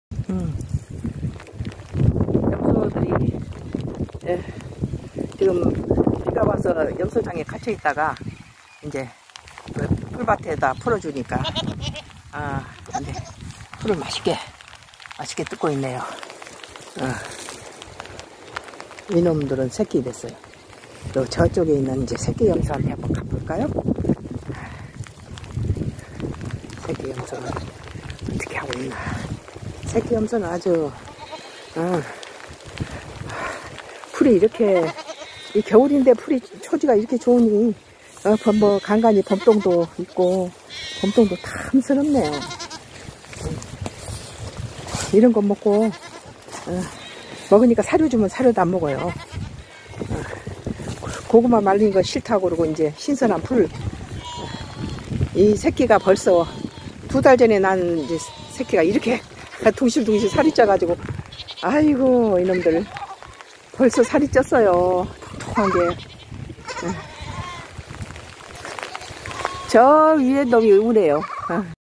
비오는날~~풀뜯으로 나온 염소 가족들~~!@sungodcross
비오는날~~풀뜯으로 나온 염소 가족들~~!

요며칠~~줄기차게 비가 내려서~~염소장 안에서~~마른 고구마잎과 호박과 사료로 배를 채우던 염소들이`~~!

오늘! 잠깐 비가 멈추자 ~~초록색 들판으로 나와서~~
초록색 풀과 봄동으로 배를 채우고 있다~~!@sungodcross